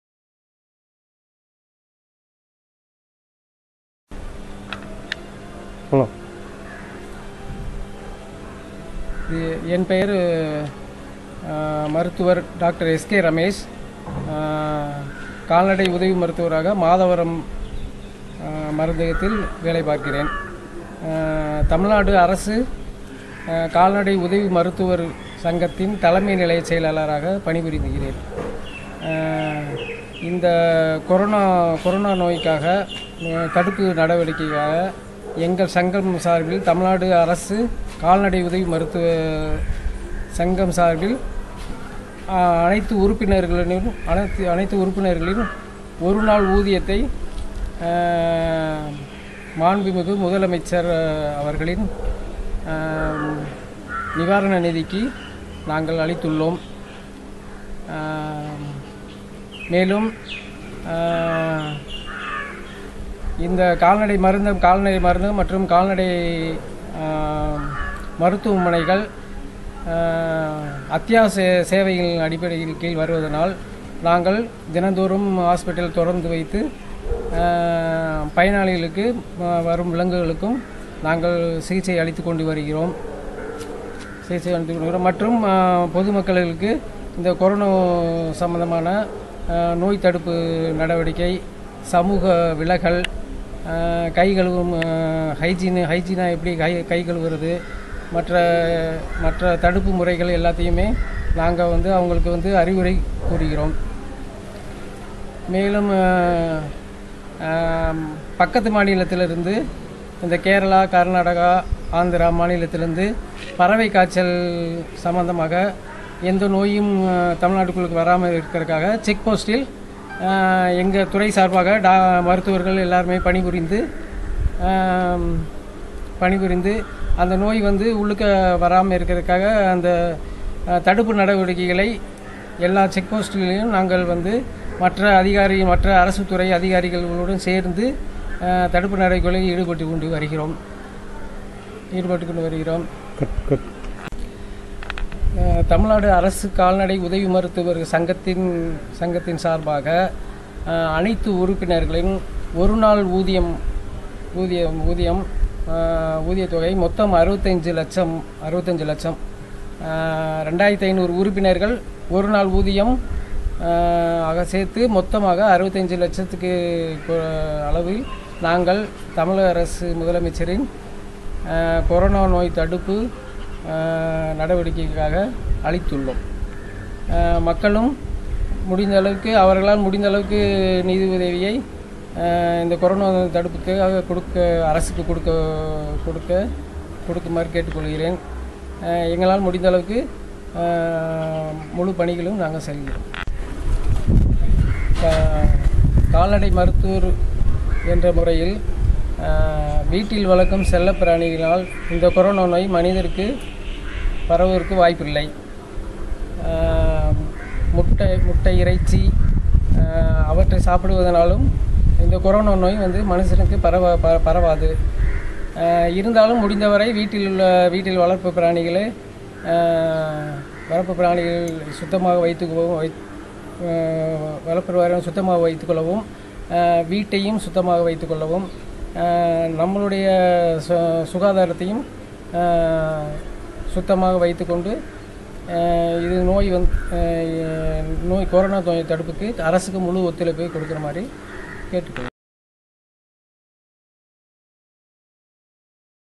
Tamilnadu Veterinary Assistant Doctors Association HQ Secretary Dr.S.K.Ramesh -corono Relief Fund
தமிழ்நாடு கால்நடை உதவி மருத்துவர்கள் சங்கம் தலைமை நிலைய செயலாளர் டாக்டர் எஸ்.கே.ரமேஷ்  பேட்டி

சென்னை தலைமை அலுவலகத்தில் பேட்டியளித்த டாக்டர் எஸ்.கே.ரமேஷ். எங்கள் சங்கத்தின் சார்பில் தமிழக முதல்வர் அறிவித்த கோரோனோ பாதிப்பு நிவாரண நிதிக்கு சங்க உறுப்பினர்கள் அனைவரும் ஒருநாள் சம்பளமான ரூ.65 லட்சத்தை அளித்துள்ளோம். மேலும் வீட்டு செல்லப்பிராணிகளால் கோரோனோ பாதிப்பு வராது எனவும், அதனை பராமரிக்கவேண்டும் என்றும் கேட்டுக்கொண்டார்.

நம் வீட்டில் சமைக்கும் இறைச்சி,முட்டைகளினால் கோரோனோ போன்ற நோய்கள் வராது எனவும் கூறினார்.